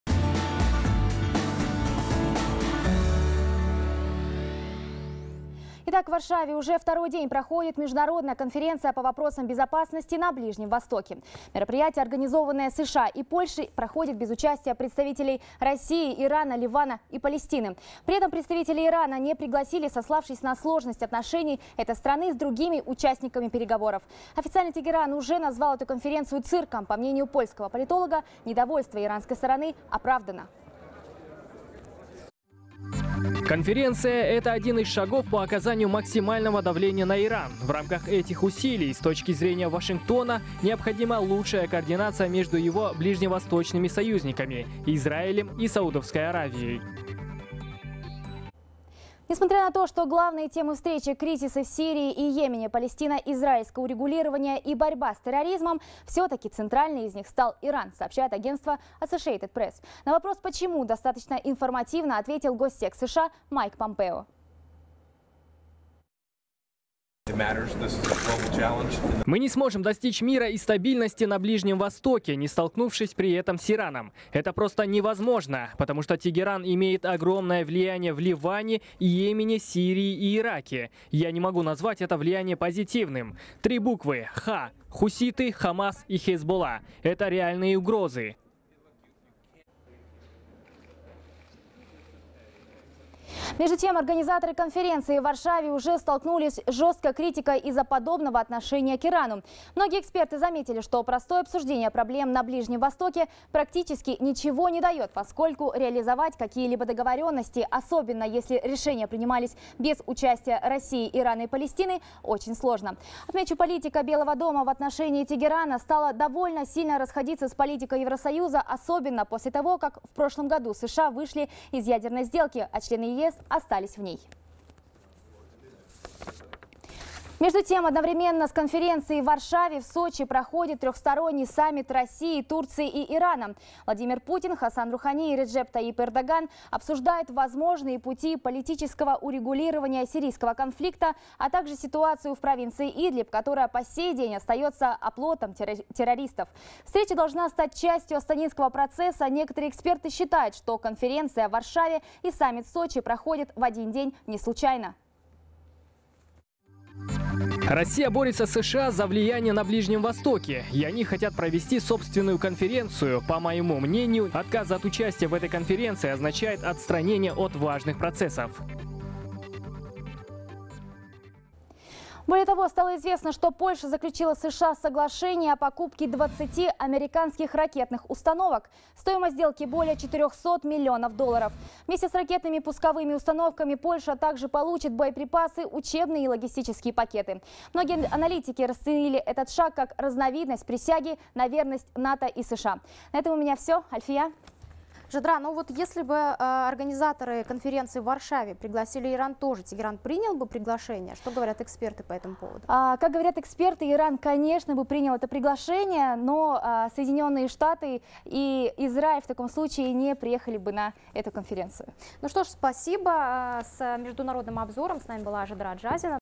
Безопасность на Ближнем Востоке обсуждают в Варшаве и Сочи
Безопасность на Ближнем Востоке - на сегодняшний день одна из самых актуальных тем в международном сообществе. По этому вопросу проходят встречи в двух разных городах: Варшаве и Сочи, сообщает корреспондент «Хабар 24».

Участники обсуждают способы урегулирования ситуаций в Йемене, Сирии и Палестине.